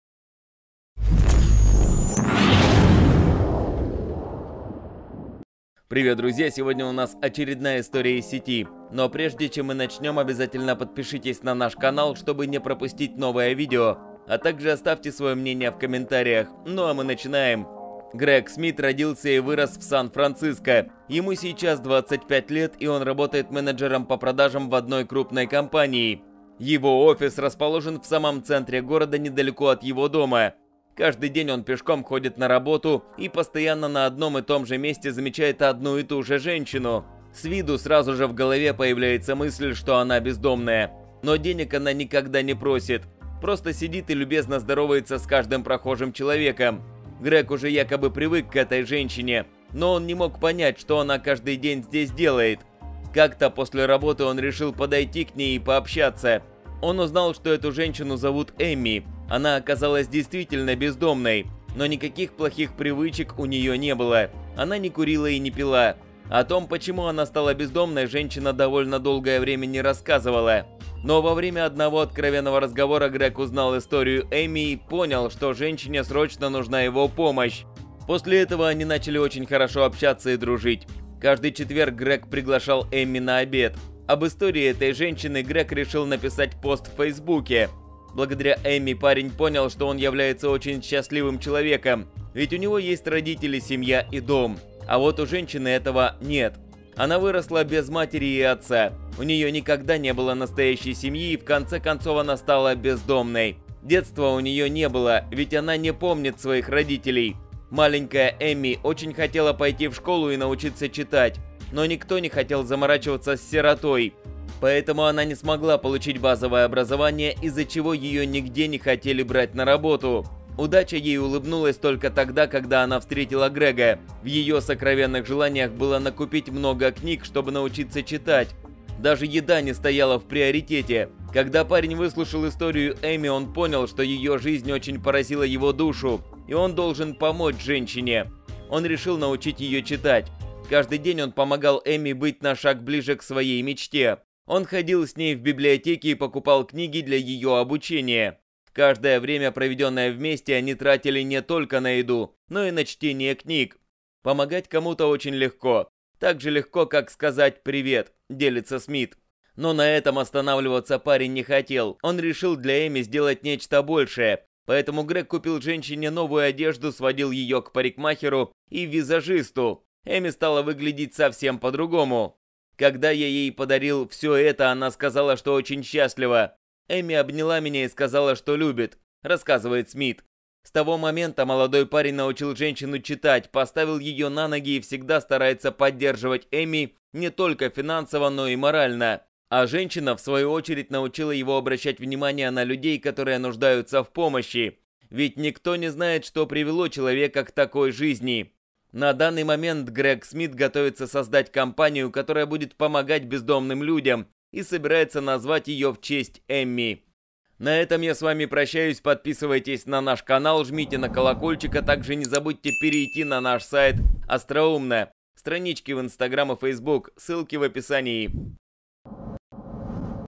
Он обедал с бездомной каждую неделю, но был поражен, ведь деньги она тратила вовсе не на еду
Грег Смит родился и вырос в Сан-Франциско. Ему сейчас двадцать пять лет и он работает менеджером по продажам в одной крупной компании. Его офис расположен в самом центре города, не далеко от его дома.